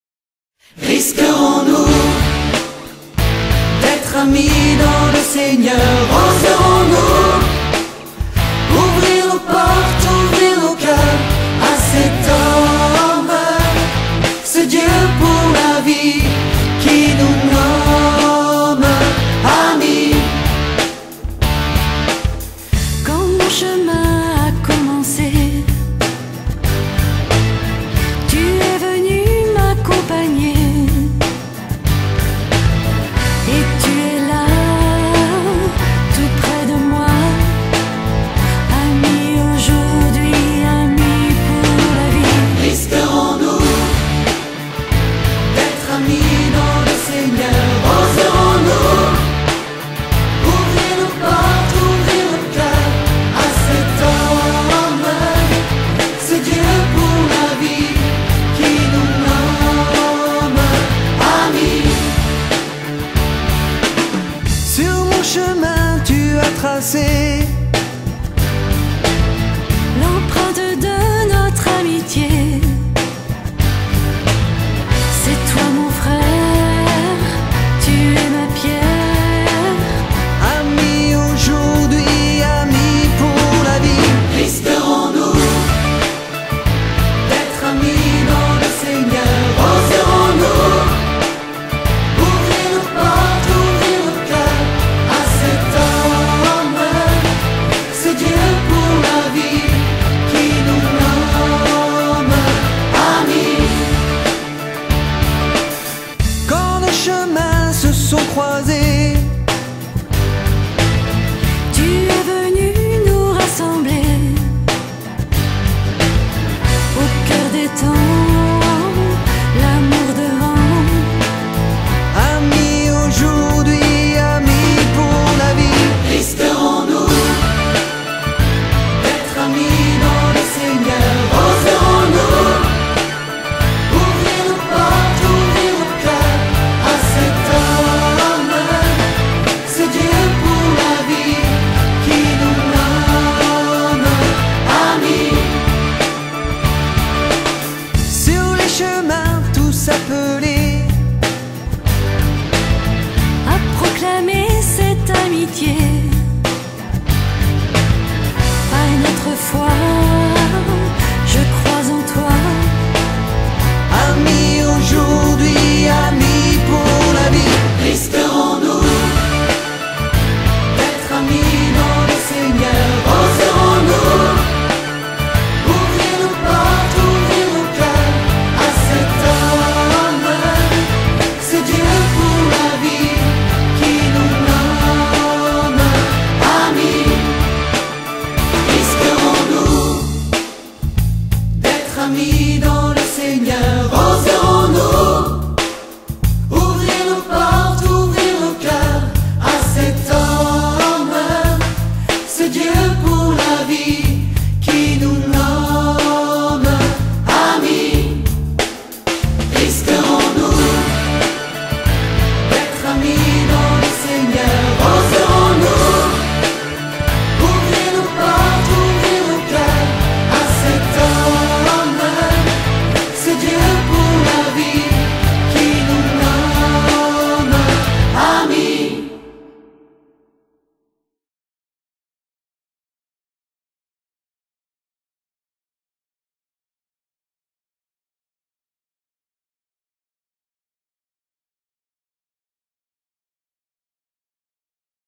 Week-end 6èmes Derval Mars 2010.wmv
Week-end de préparation à la profession de foi, les 6 et 7 Mars 2010 pour les 6èmes des paroisses de Rezé, Les Sorinièes, Pont-Saint-Martin et les Couëts